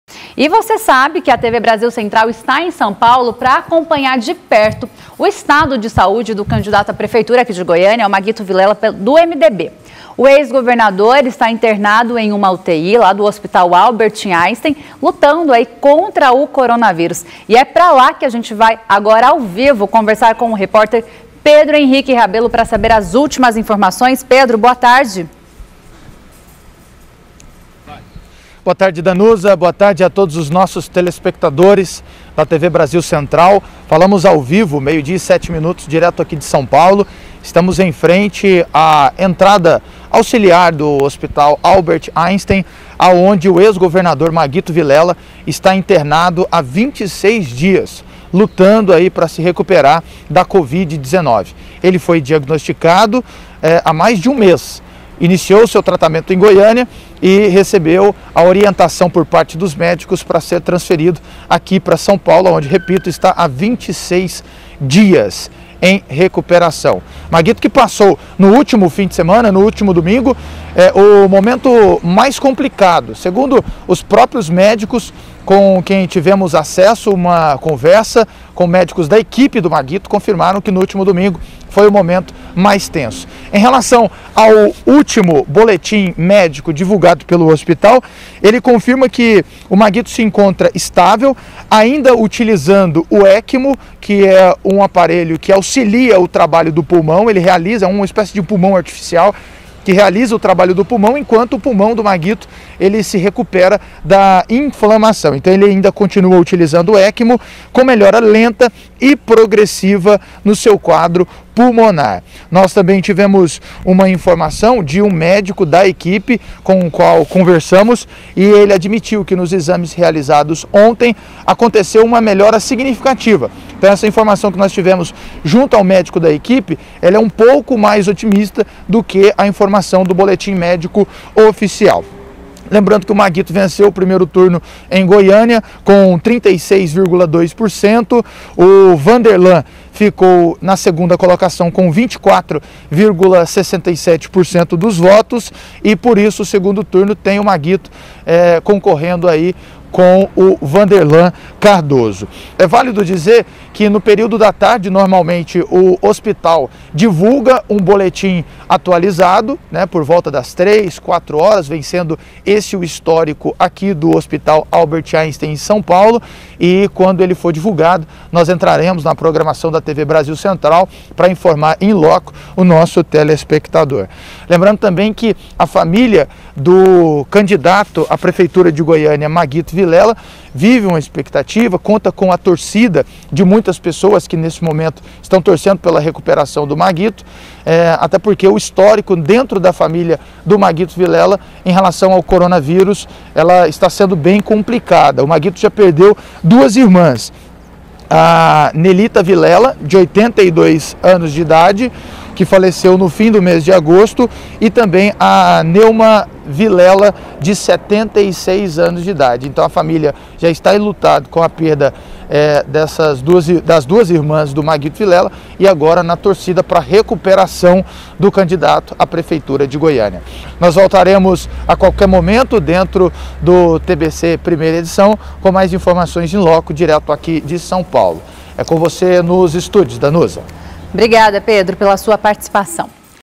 EXCLUSIVO DE SÃO PAULO | CONFIRA COMO ESTÁ O ESTADO DE SAÚDE DE MAGUITO VILELA
Data de veiculação: 21/11/2020
Apresentadores: Danuza Azevedo
Repórter: Pedro Henrique Rabelo
Imagens: Renato Gonçalves